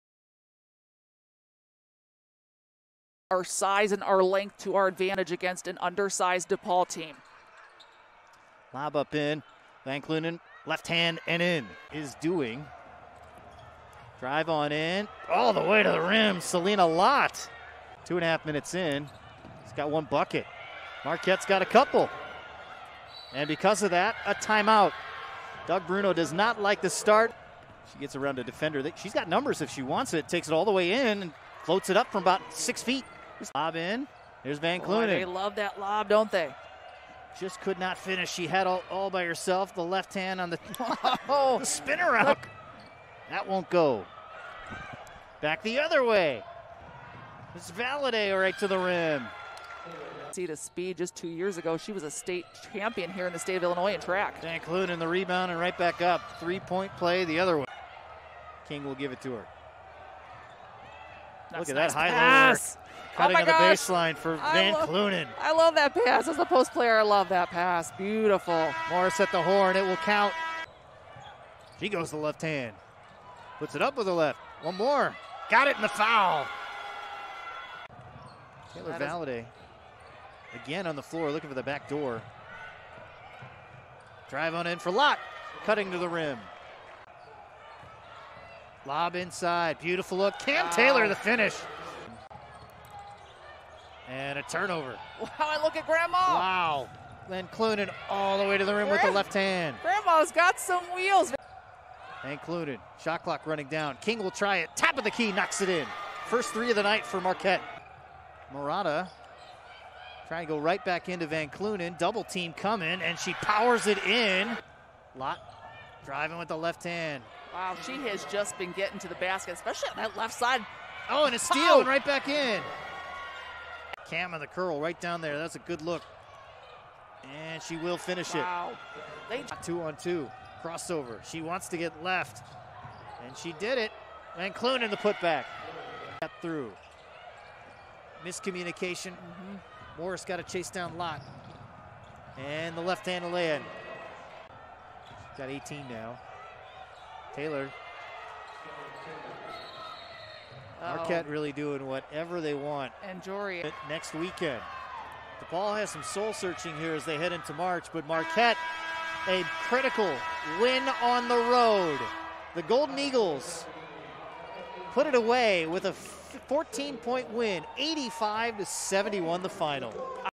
Highlights: Marquette Women's Basketball 85, No. 24/23 DePaul 71 | 2.24.21
The Golden Eagles moved to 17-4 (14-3 BIG EAST) with a 14 point win over No. 24/23 DePaul. Selena Lott had 25 points to lead MU and the Golden Eagles clinched the No. 2 seed in the BIG EAST tournament.